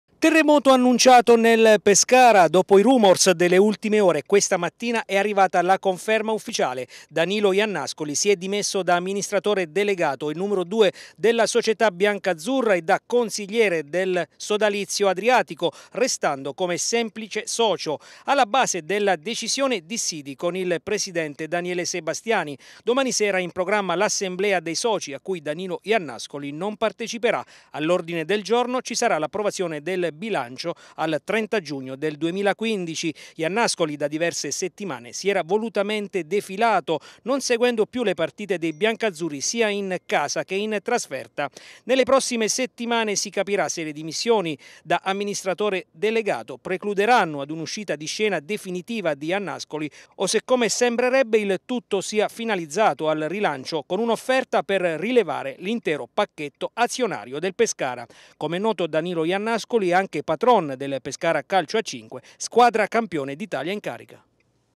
Pescara calcio. Iannascoli si dimette
Terremoto in società. Questa mattina sono infatti arrivate le dimissioni dell'ad Danilo Iannascoli che resta come semplice socio. Domani l'approvazione del Bilancio nel Consiglio di Amministrazione